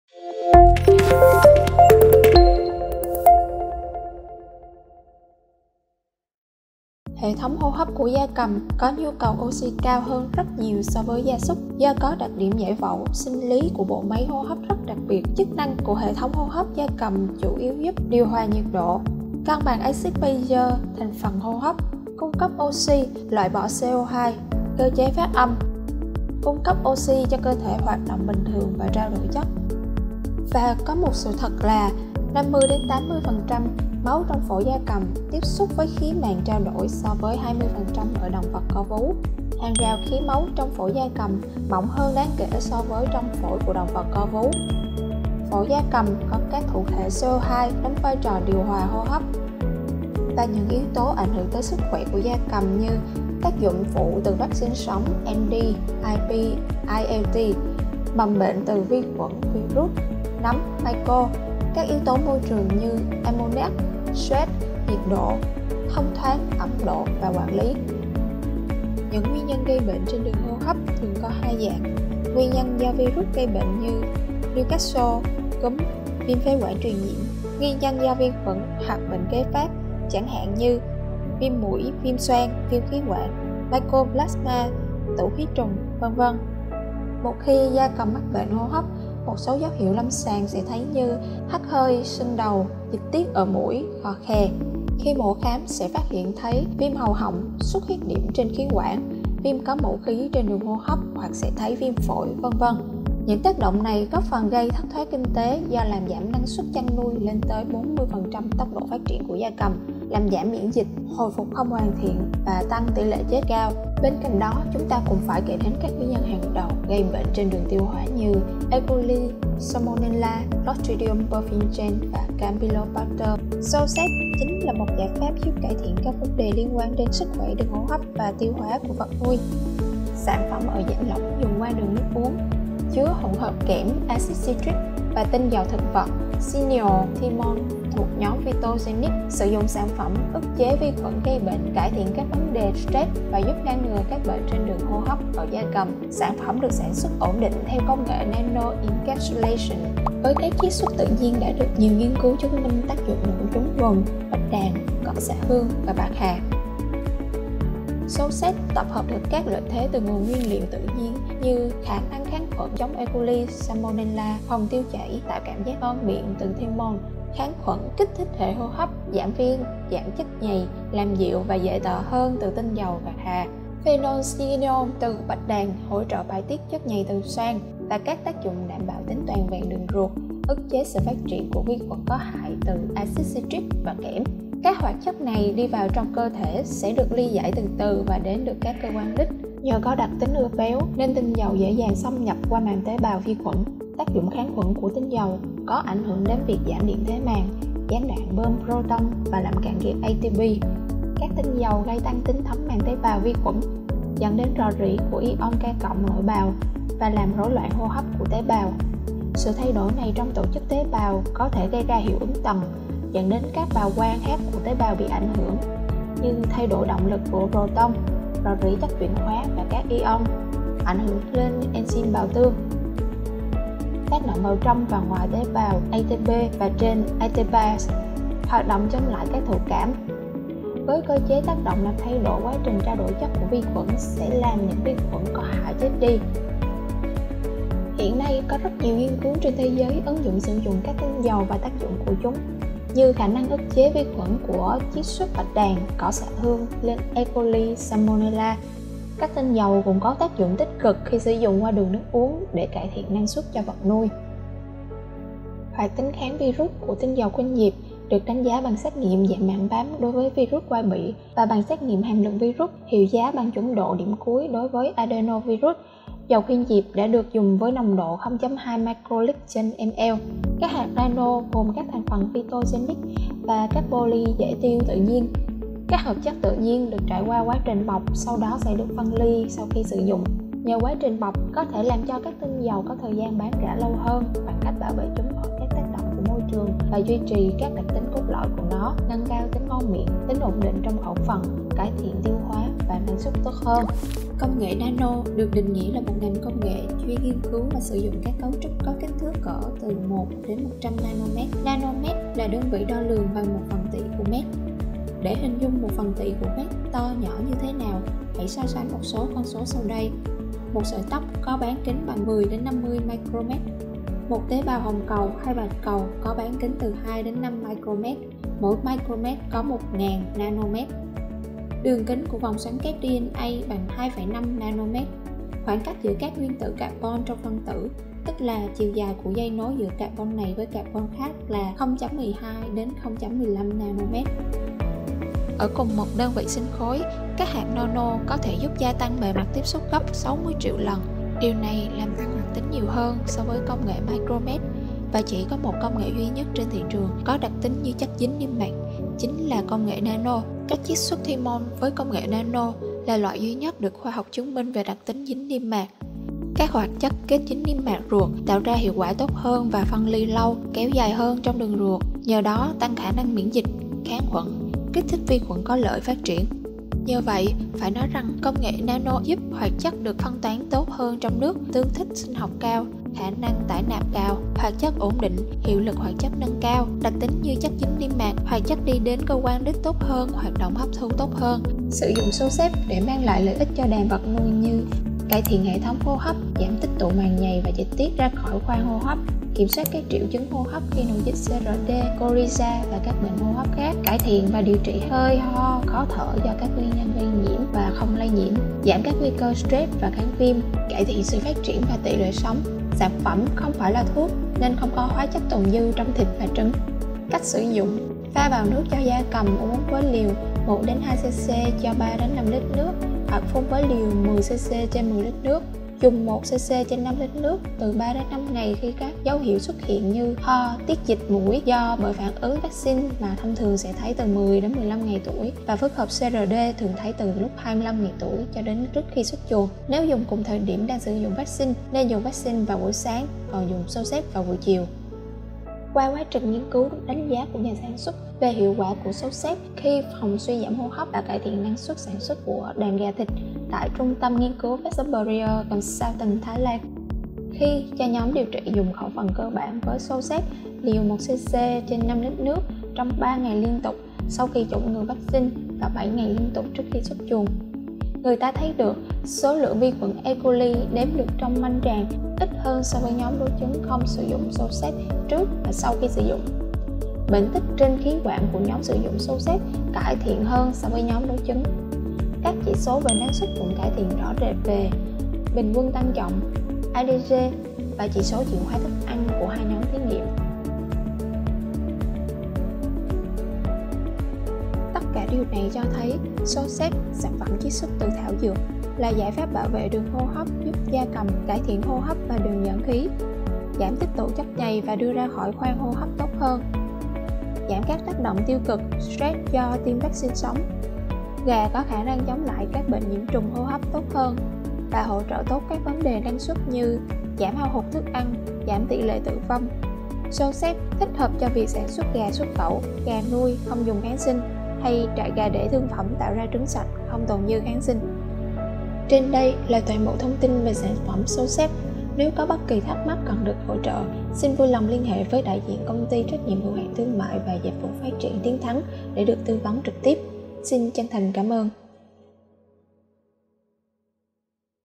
XO SAVE giải pháp thay thế kháng sinh phòng bệnh hô hấp và tiêu hóa trên gia cầm
XO-SAVE là dung dịch chứa hỗn hợp khoáng, chiết xuất thực vật hòa tan trong nước uống vật nuôi. Trong 1lít Xo save chứa Kẽm, Citric acid, Tinh dầu khuynh diệp, Thymol.
Sử dụng XO-SAVE giúp cải thiện hô hấp và lưu thông khí cho gia cầm.
Fanpage:tienthangvet